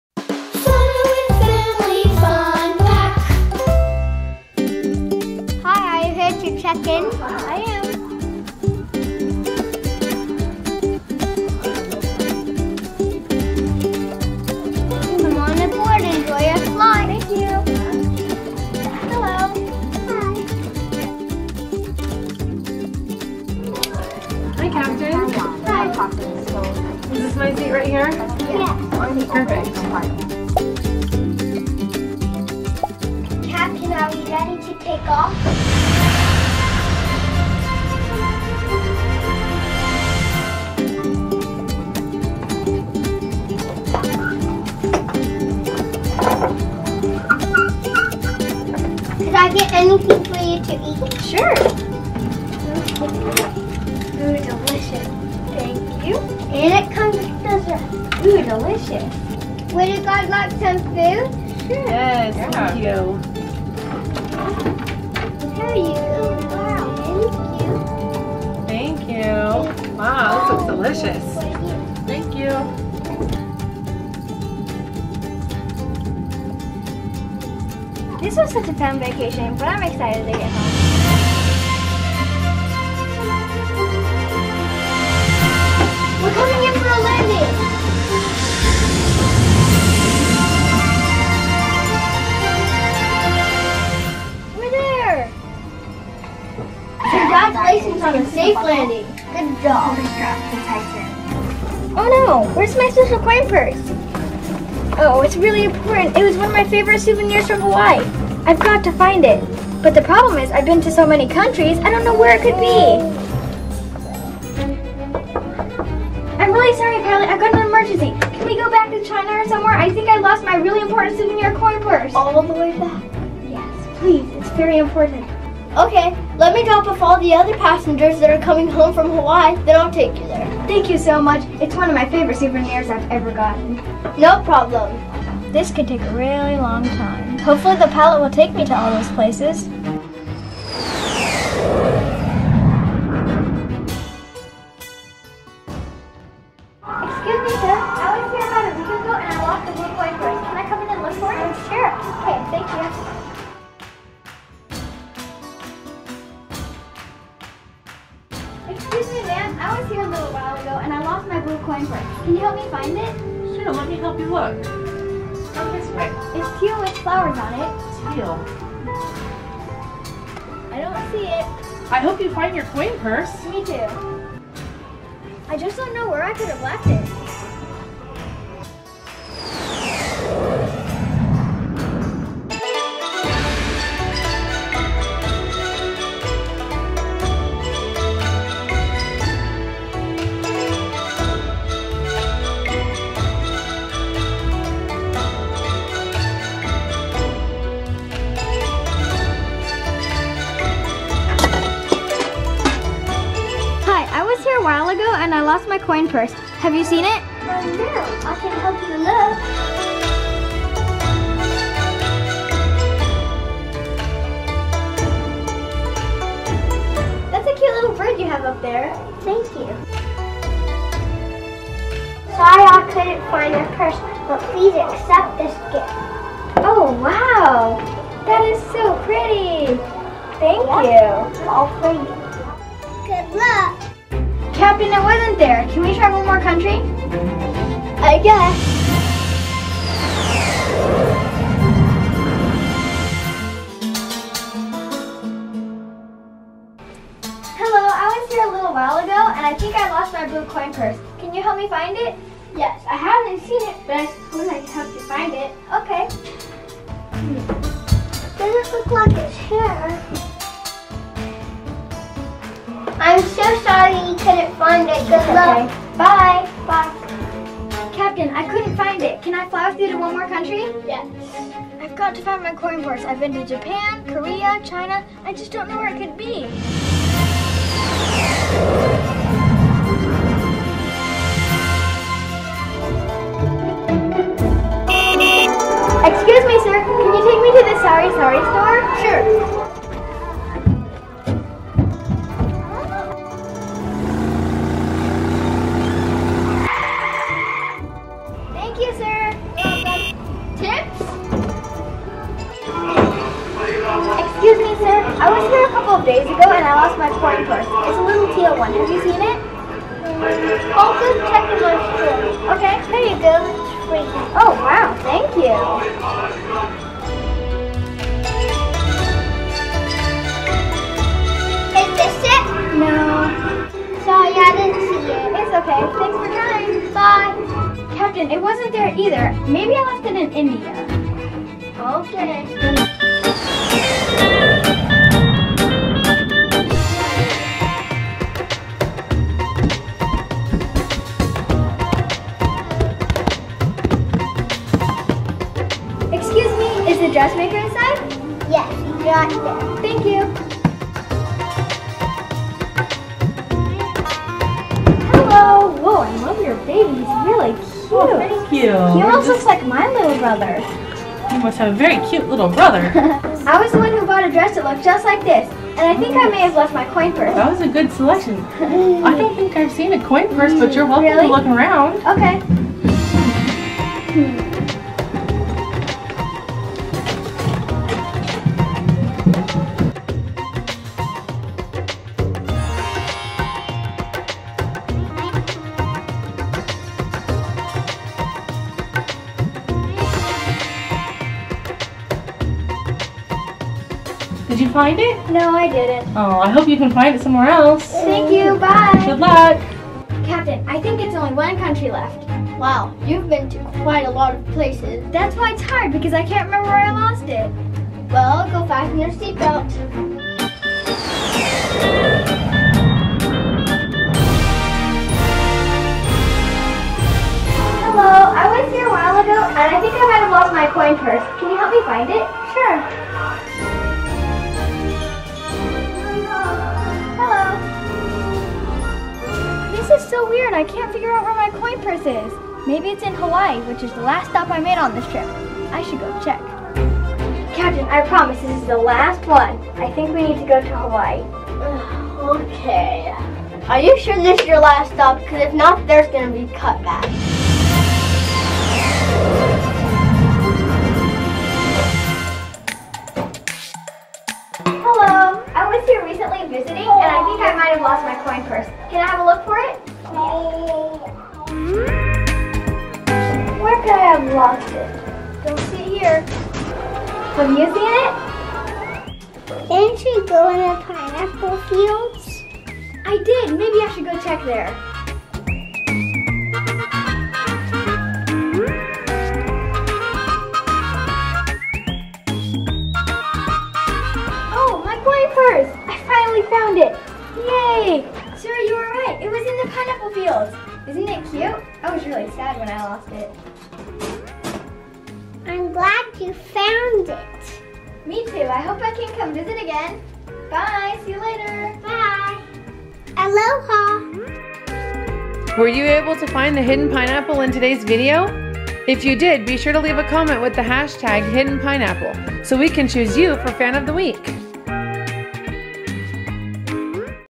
Alyssa's Missing Purse! Kid Pilot Saves the Day!
Alyssa's Missing Purse! Kid pilot saves the day! Oh no, what happened to Alyssa's special coin purse souvenir?? Pilot David flies Alyssa all over the world to look for her lost purse. Along the way she visits India, Portugal, China, Japan, Korea, the Philippines, and of course Hawaii in the United States.  We hope you love this skit as much as we loved making it, and you might even learn a bit about world geography and culture as you watch it!